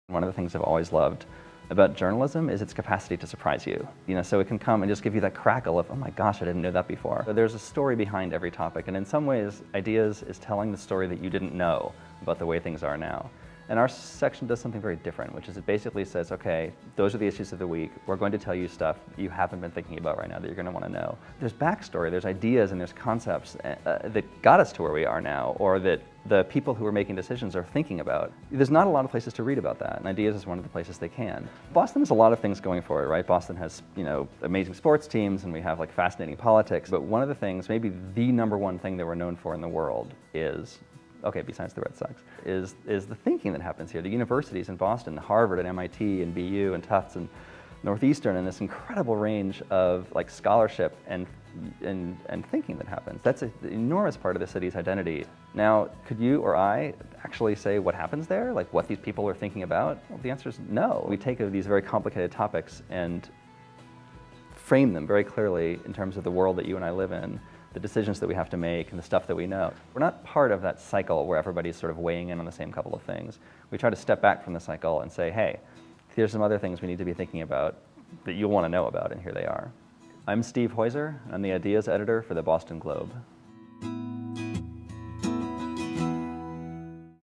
The Boston Globe Journalist Series: Steve Heuser
An interview with Steve Heuser, a Boston Globe Ideas Editor.